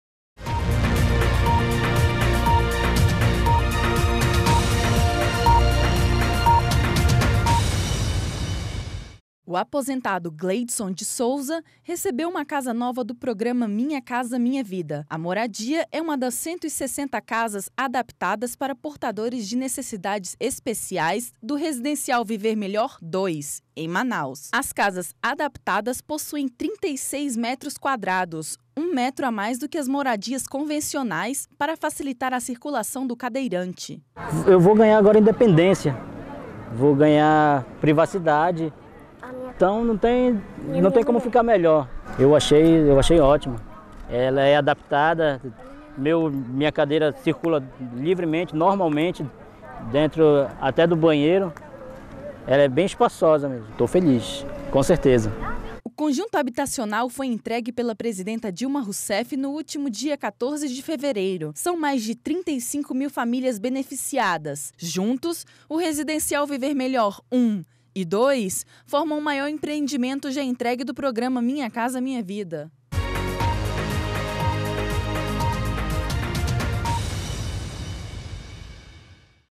Gleidson Viana de Souza - Depoimento de beneficiário do MCMV em Manaus
Cerca de 21 mil pessoas foram beneficiadas, no dia 14 de fevereiro, em Manaus (AM), com a entrega das 5.384 unidades do residencial Viver Melhor 2 pelo Programa Minha Casa Minha Vida (MCMV). Os empreendimentos, destinados a famílias com renda de até R$ 1,6 mil (Faixa 1), receberam investimento no total de R$ 313,39 milhões, com recursos do Fundo de Arrendamento Residencial (FAR). Gleidson de Souza foi um dos beneficiados que recebeu uma casa totalmente adaptada para pessoas com dificuldade de locomoção. Assista ao depoimento e compartilhe: